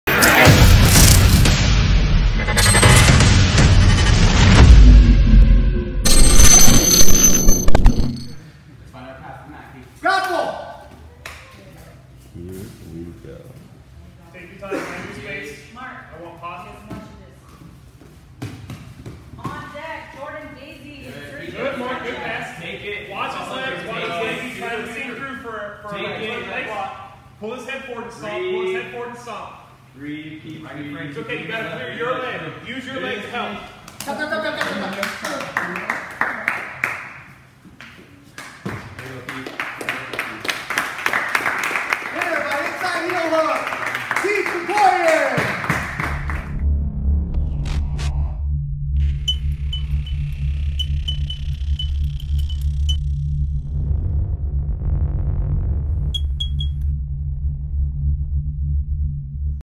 Keith Krikorian vs Mark Wilson Bullpen submission series purple belts sub via heel hook
Bullpen SUbmission Series
January 27, 2018
Purple Belts

155 Division
Round 1
Keith wins via inside Heel Hook in :24sec

Location
10th planet tucson
Tucson arizona